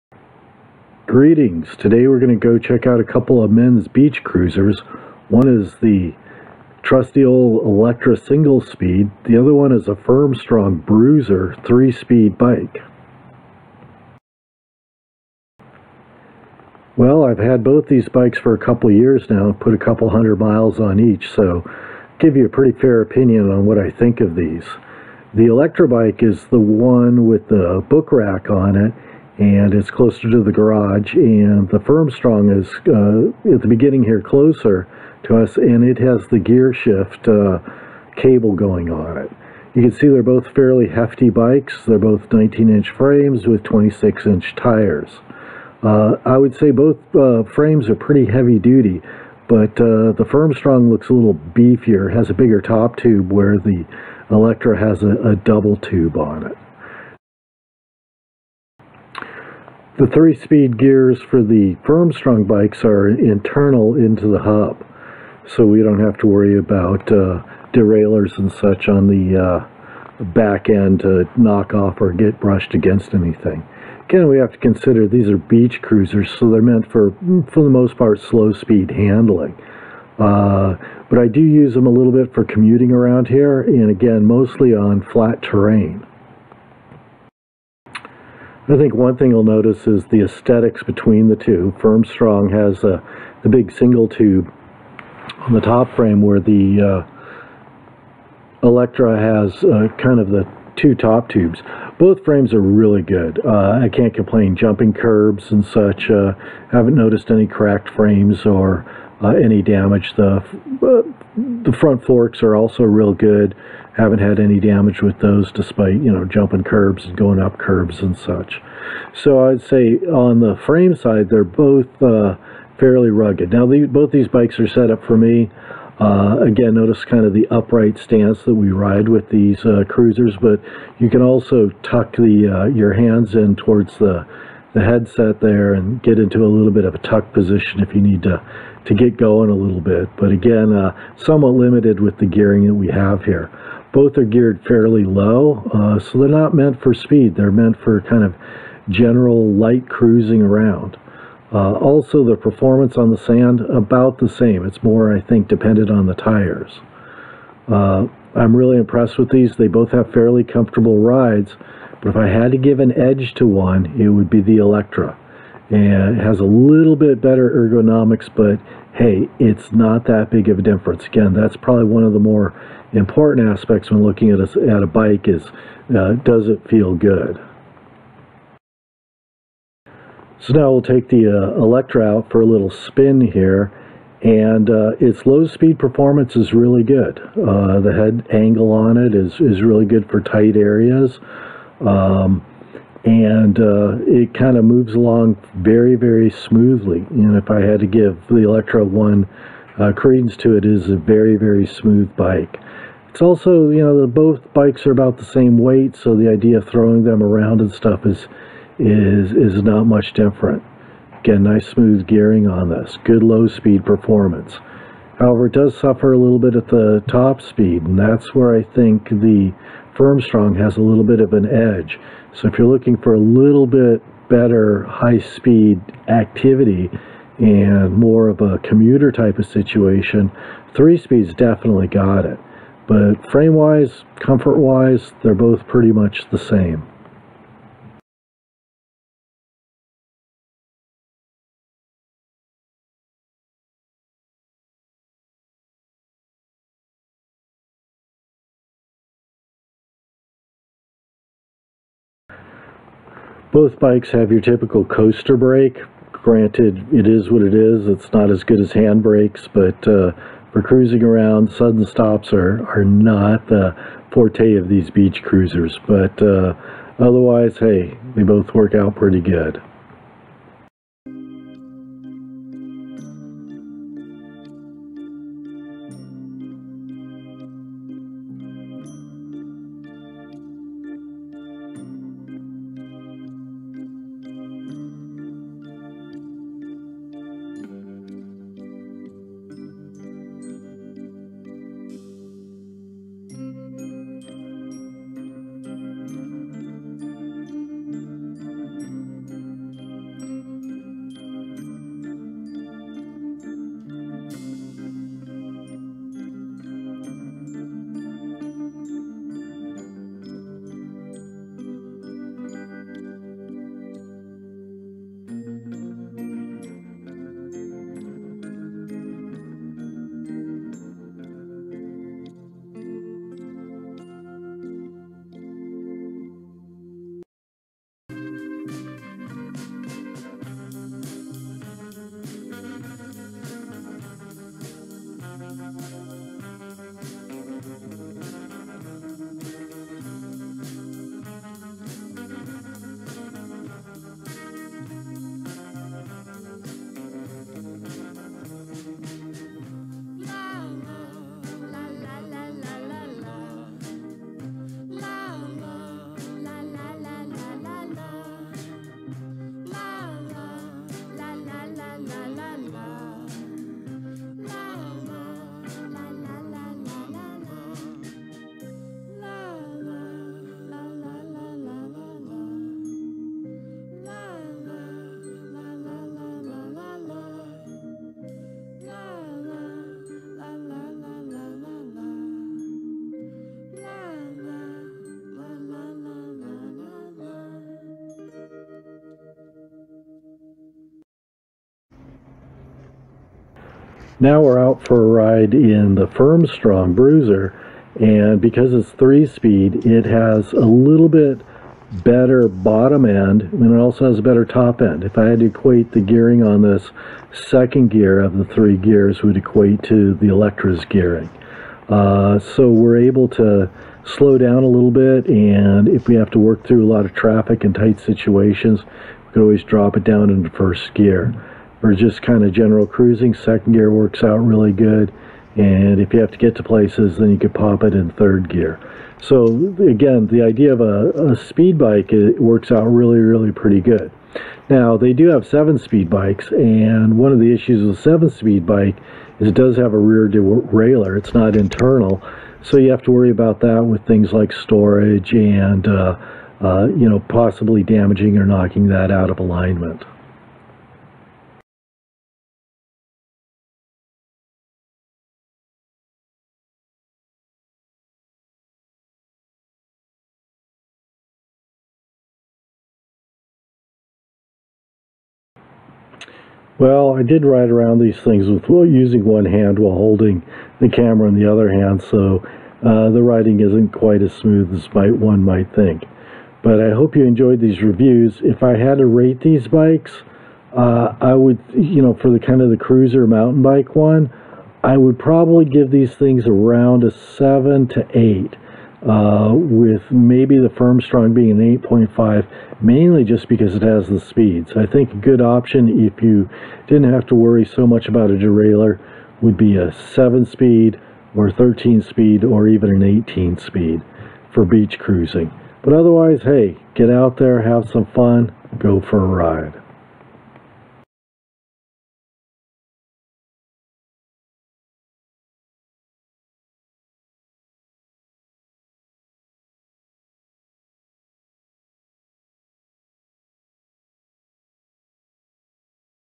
Bike Review: Electra Single Speed vs Firmstrong Bruiser 3 Speed
I have had these two men's beach cruiser bicycles for some time and thought I'd compare and give my opinion on both these. The Electra and Firmstrong are both good bikes but because the Firmstrong has 3 gears, it will get top honors.